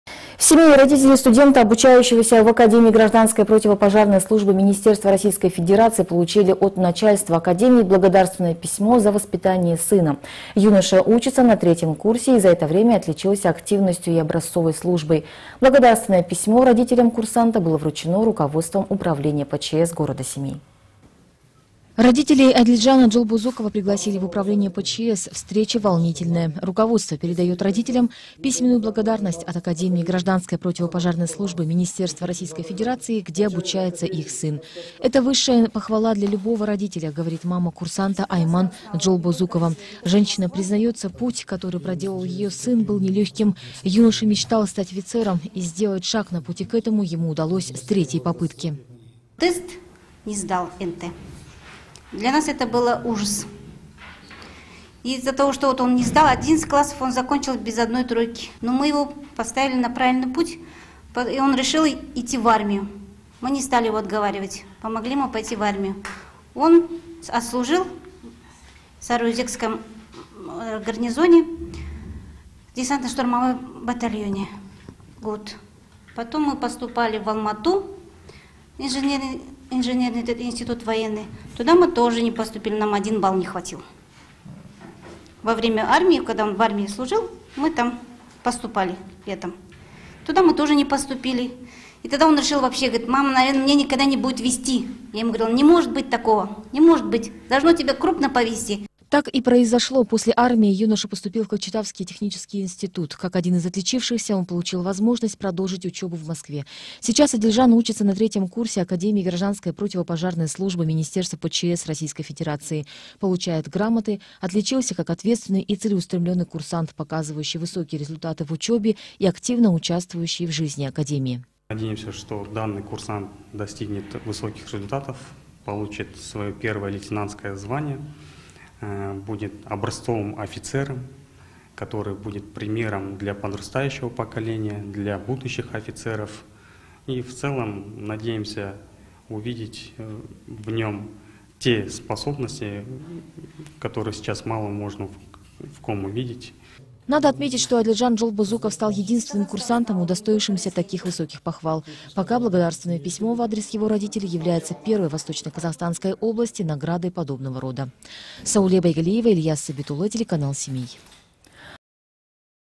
БЛАГОДАРНОСТЬ ИЗ РОССИИ
В Семее родители студента, обучающегося в Академии гражданской противопожарной службы Министерства Российской Федерации, получили от начальства Академии благодарственное письмо за воспитание сына. Юноша учится на 3 курсе и за это время отличился активностью и образцовой службой. Благодарственное письмо родителям курсанта было вручено руководством Управления по ЧС г.Семей.